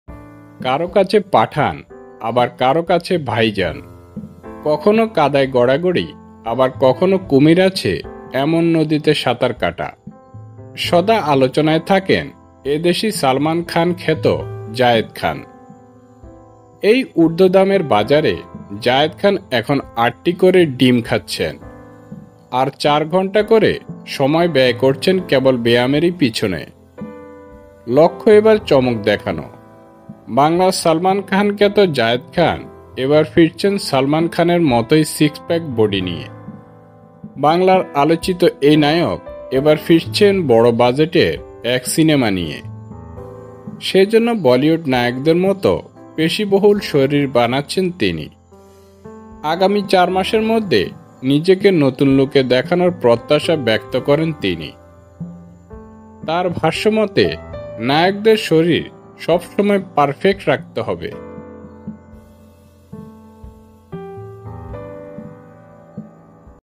কেন প্রতিদিন ৮টি করে ডিম খাচ্ছেন জায়েদ খান ? | Zayed Khan Interview
কারো কাছে পাঠান আবার কারো কাছে ভাইজান! কখনো কাদায় গড়াগড়ি আবার কখনো কুমির আছে এমন নদীতে সাতার কাটা। সদা আলোচনায় থাকেন এই দেশী সালমান খান খ্যাত জায়েদ খান!